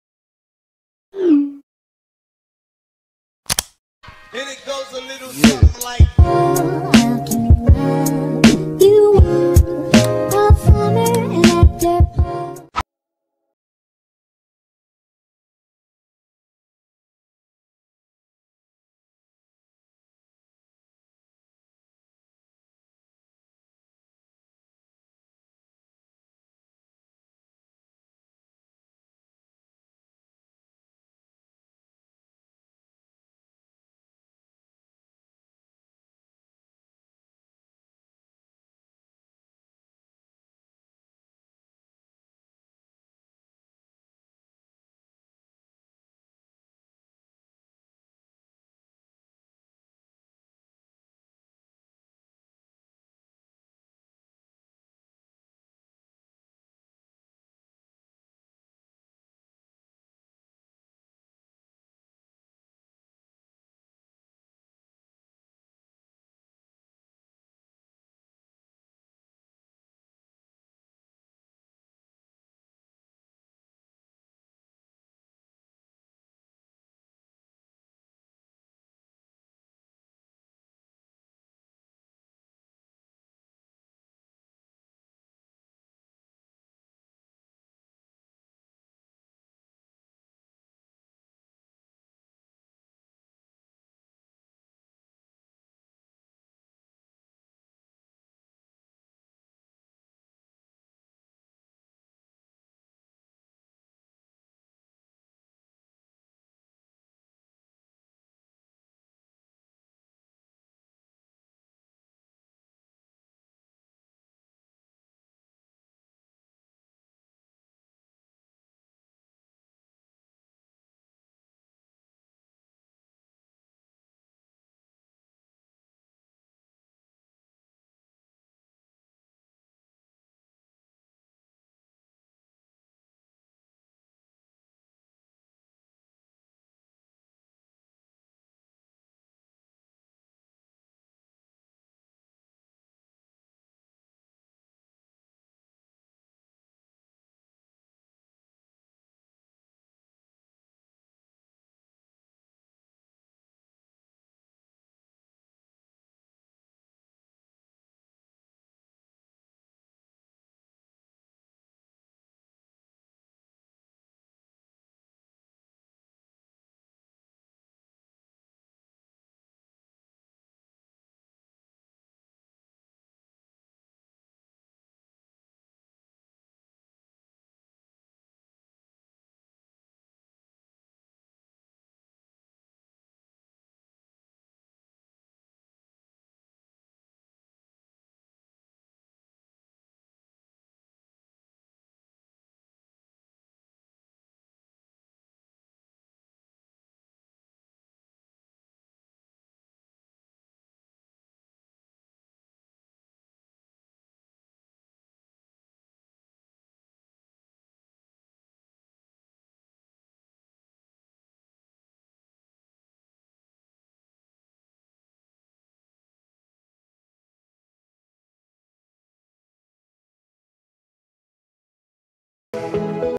•𝗘𝗮𝘀𝘆 𝗕𝗹𝗲𝗻𝗱𝗶𝗻𝗴 𝗘𝗱𝗶𝘁𝗶𝗻𝗴 𝗧𝘂𝘁𝗼𝗿𝗶𝗮𝗹 𝗳𝗼𝗿 𝗳𝗮𝗻𝗽𝗮𝗴𝗲𝘀• ||• Hiti's Creation•
❥Hey Guys, it's me Hiti here
❥Hope you all are safe and well.So, in today's video I'm gonna show you how to make beautiful and easy aesthetic grid  edit with using amzing apps

•Thank you so much for coming here•

[ Igonre this ]